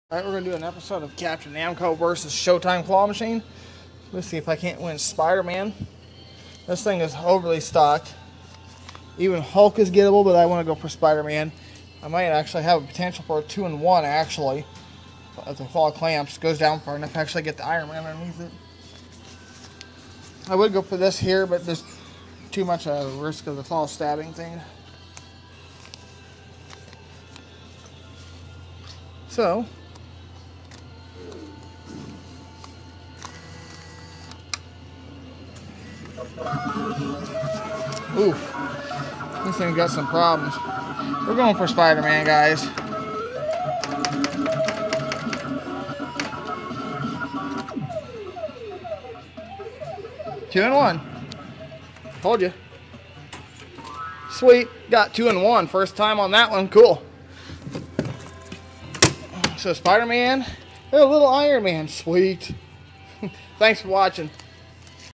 CAPTAIN NAMCO VS SHOW TIME CLAW MACHINE EPISODE 5 2 IN 1 SET UP NAILED
THIS WAS FILMED YESTERDAY AT MY LOCAL MALL HERE I FIND THAT IT DOENST LOOK LIKE NOTHING HAS BEEN WON WHEN I WALKED IN IF IT HAS PAID OUT IT IS VERY LITTLE PRIZES PAID OUT  HERE I QUICKLY NOTICE THAT THERE WAS A POTENTIAL FOR 1 2 IN1 WIN  SO I HAD TO ATTEMPT IT  AND I WAS ABLE TO PULL IT OFF